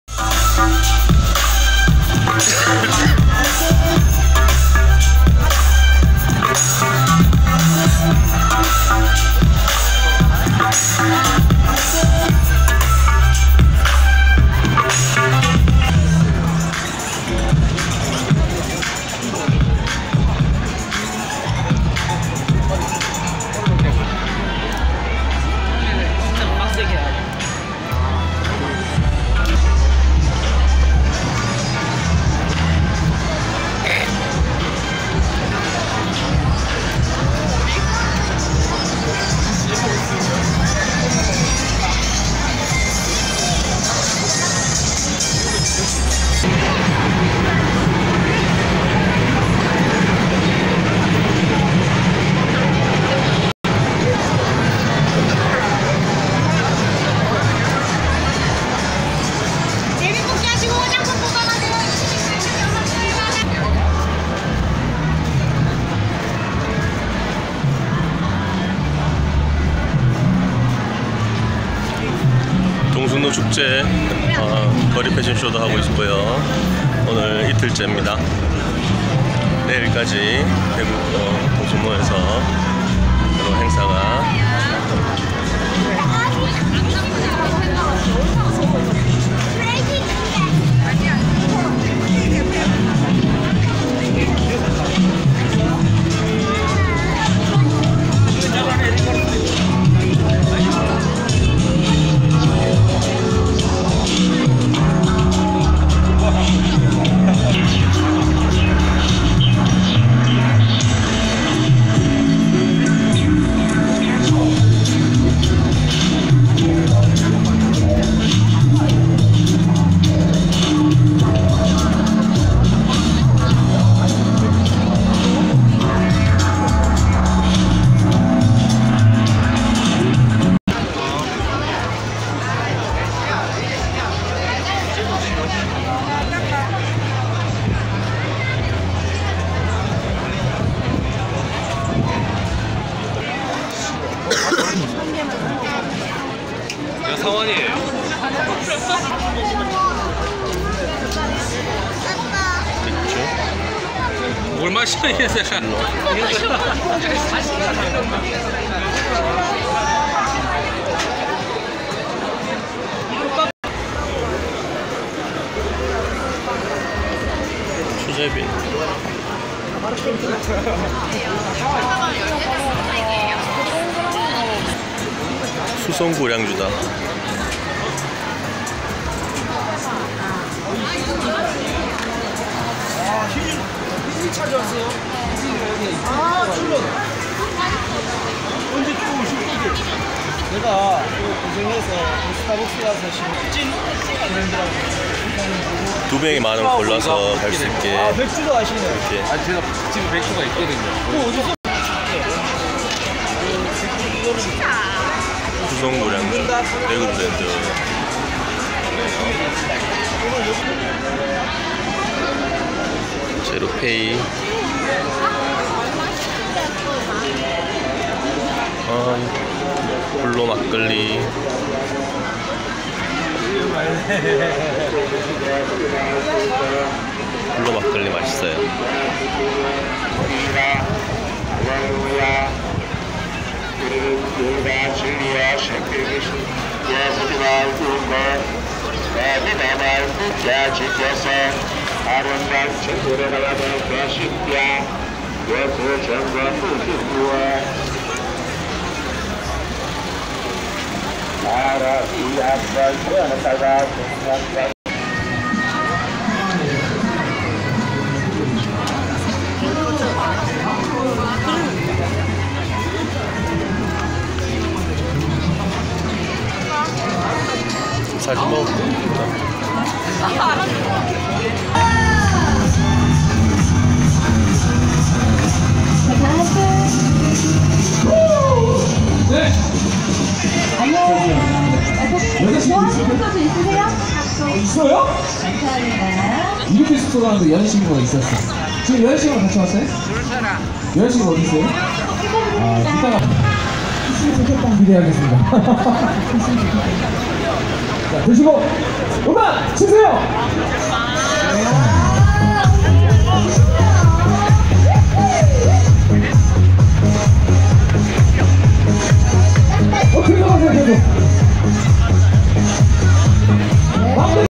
KOREA VLOG ★대구 동성로축제 19.5.11
동성로축제 거리풍경..패션쇼.무예타이★★★